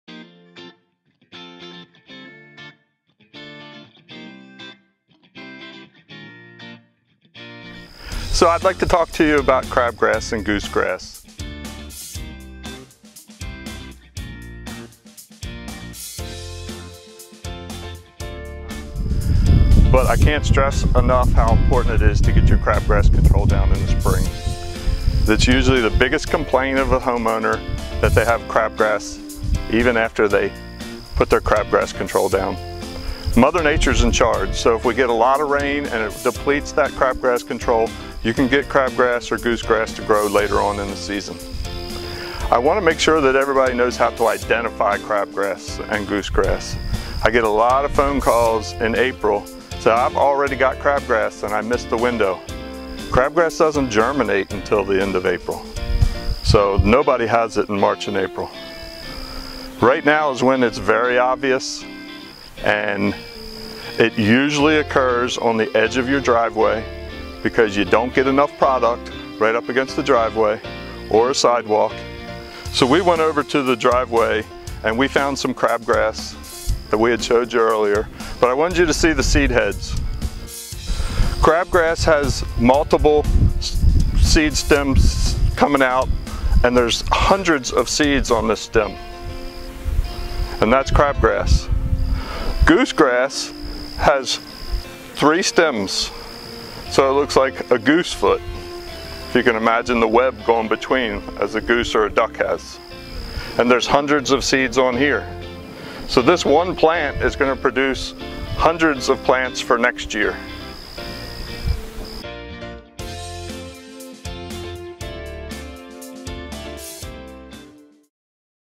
Crabgrass & Goosegrass
The Mill's turf expert discusses crabgrass and goosegrass.